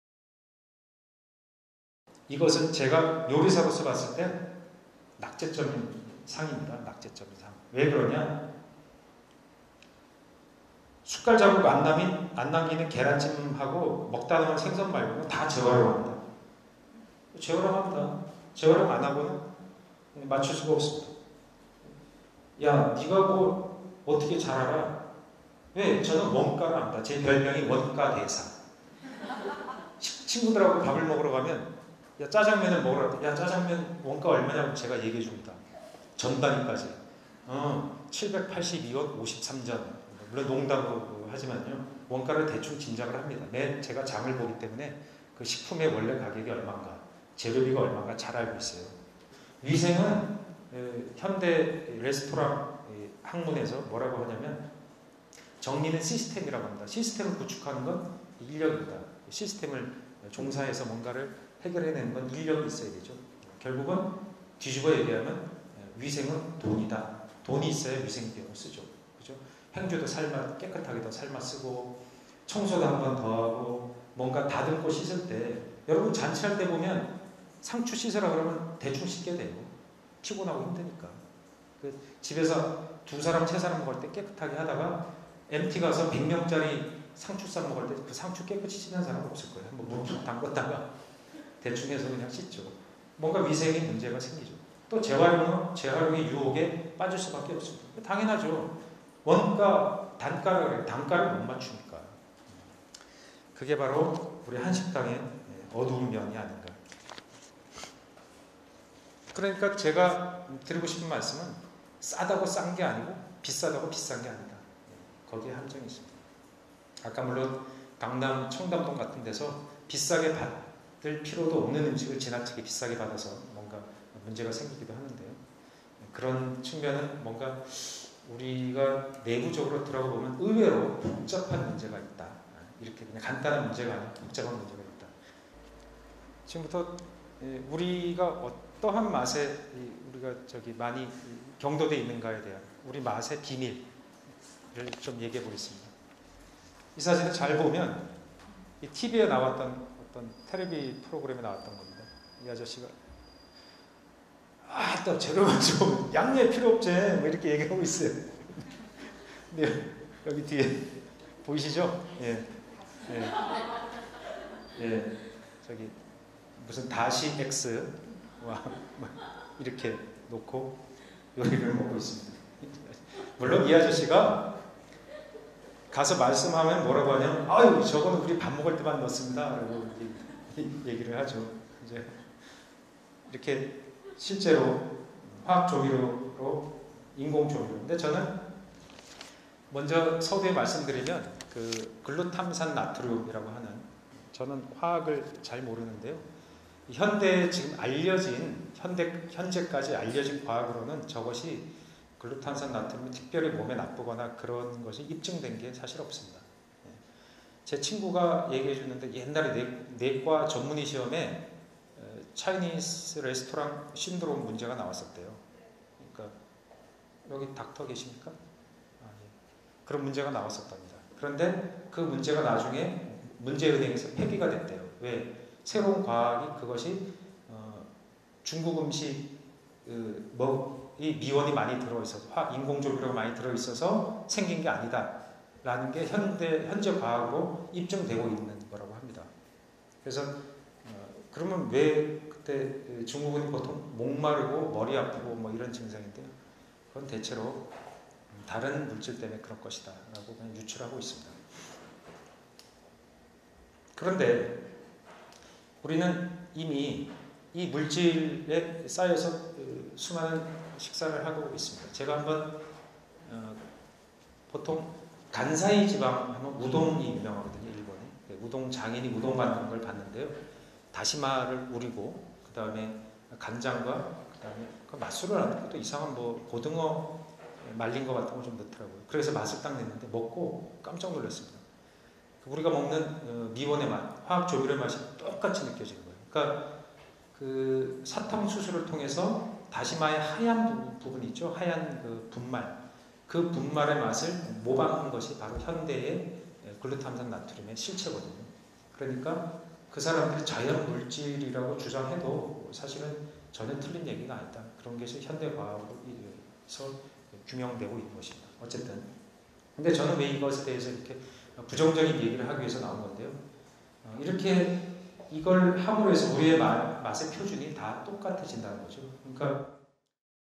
[알파레이디 북토크]박찬일 셰프 '인생은 맛이다' ③
9월의 알파레이디 북토크에서는 스타 셰프 박찬일씨가 강연자로 나섰습니다. 잡지기자 생활을 하다 요리사로 변신한 독특한 이력을 가진 그는 최근 〈추억의 절반은 맛이다〉라는 책을 출간했습니다. '인생은 맛이다'라는 주제로 열리는 이번 강연에서는 인생을 곱씹어보는 음식과 맛의 세계로 떠나봅니다.